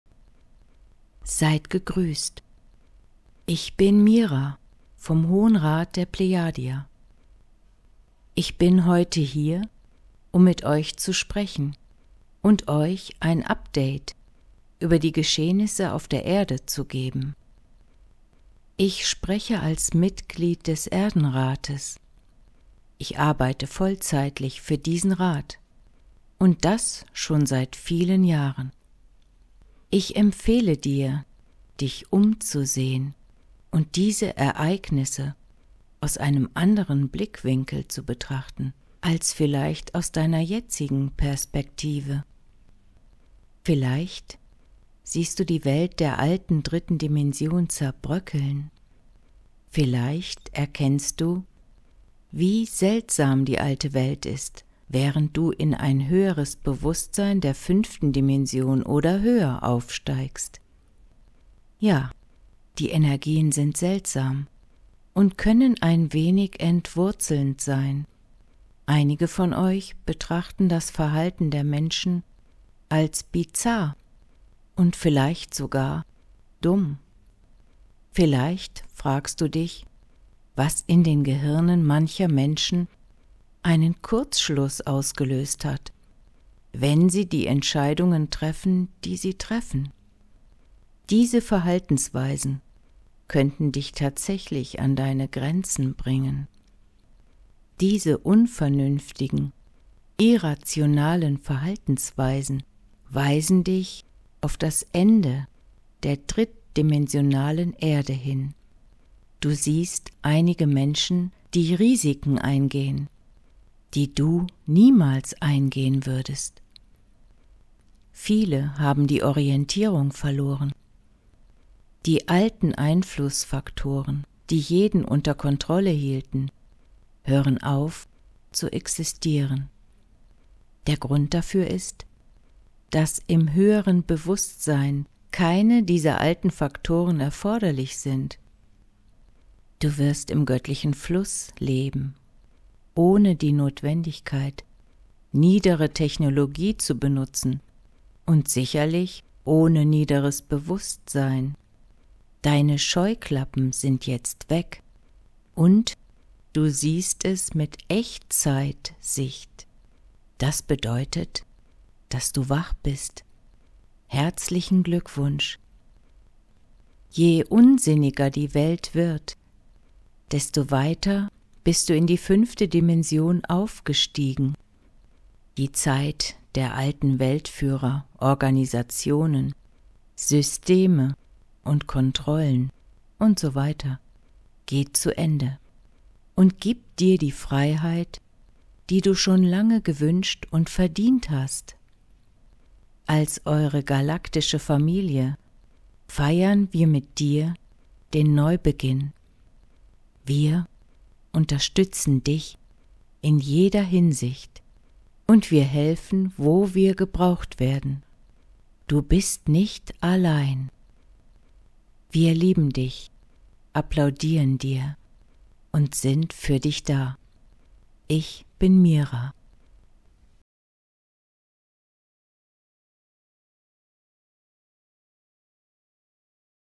Mira vom Hohen Rat der Plejadier über Valerie Donner: Update über die Geschehnisse auf der Erde
Illustrationen: Anastasiya Badun, Brian Brxtn, Dean Enkelaar, Dwinande Nurhanif Mujito, Jamie Street, Peter Herrmann, Robson Hatsukami, Simon Zhu, Victor Hesse u.a.